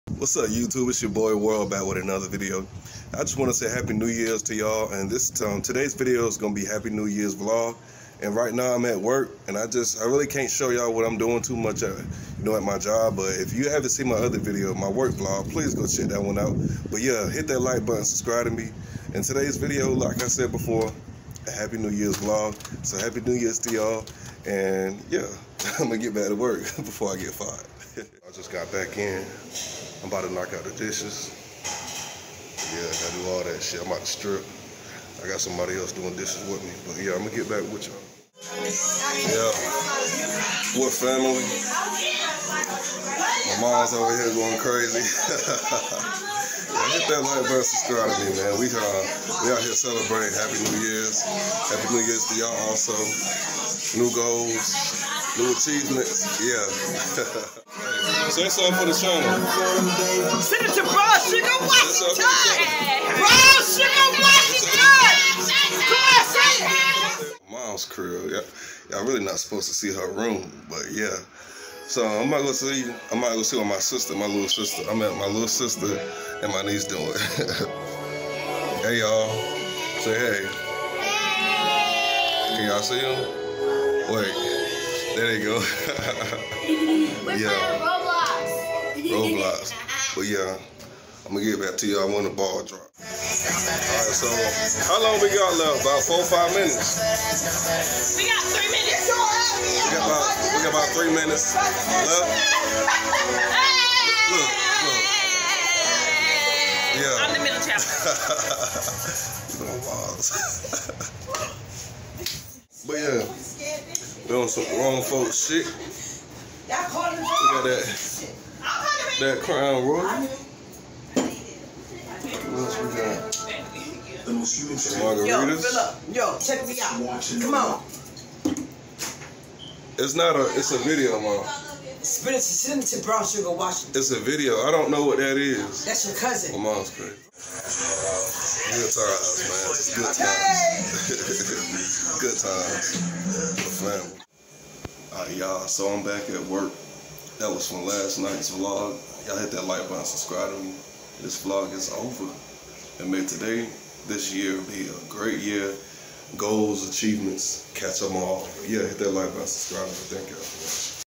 HAPPY NEW YEARS VLOG!!!! | HELLO 2021‼️‼️
what is us up Humble Nation it's ya boy World back with another video. please hit that like button and subscribe to my channel. im closing 2020 out with my family, I hope yall enjoy and apologize for the bad camera quality...HAPPY NEW YEARS!!‼️‼️😁😁😁😁😁